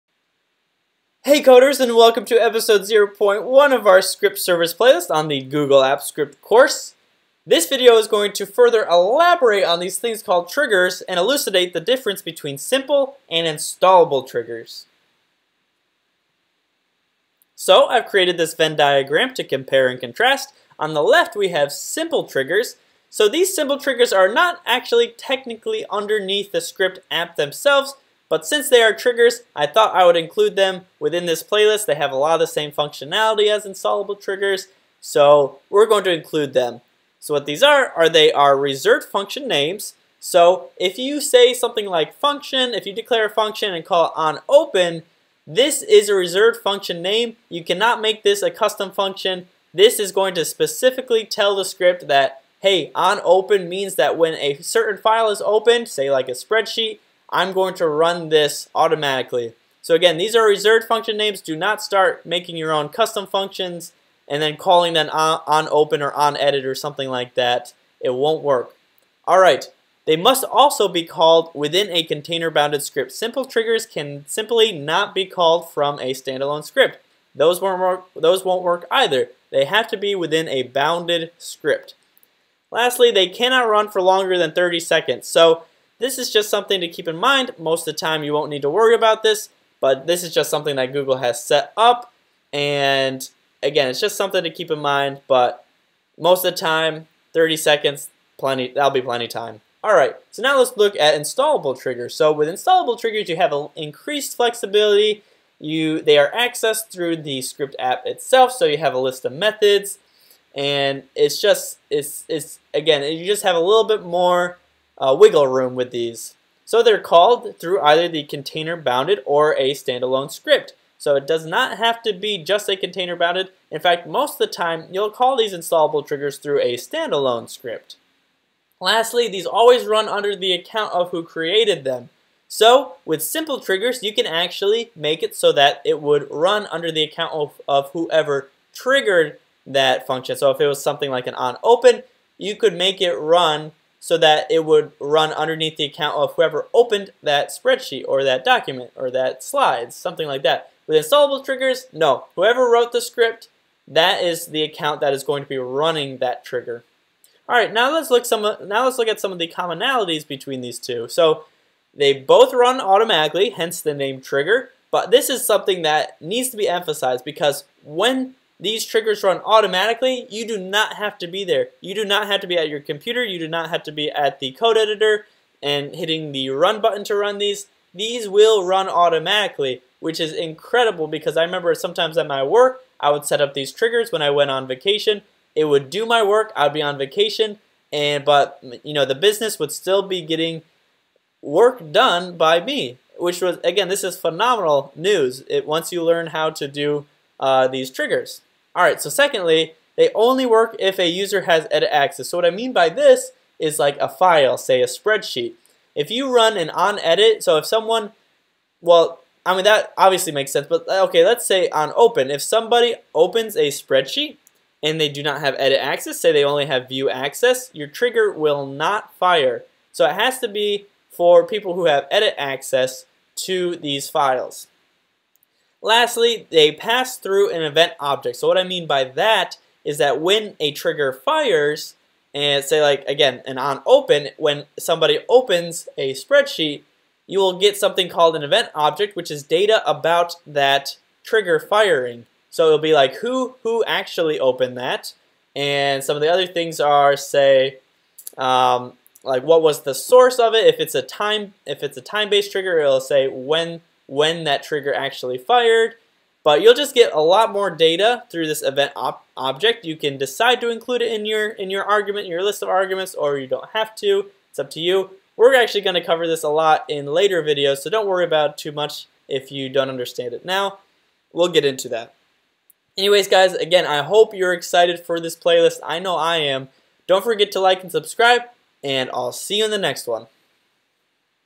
Simple vs. Installable Triggers - Episode 3.0.1 | Apps Script ~ Script Service
Hey Coders! There are two main categories of Apps Script Triggers: Simple and Installable Triggers (There's nothing to install with installable triggers, by the way!) Although simple triggers are not technically apart of the ScriptApp, they are still extremely powerful tools. Learn the key differences between these two categories in this video!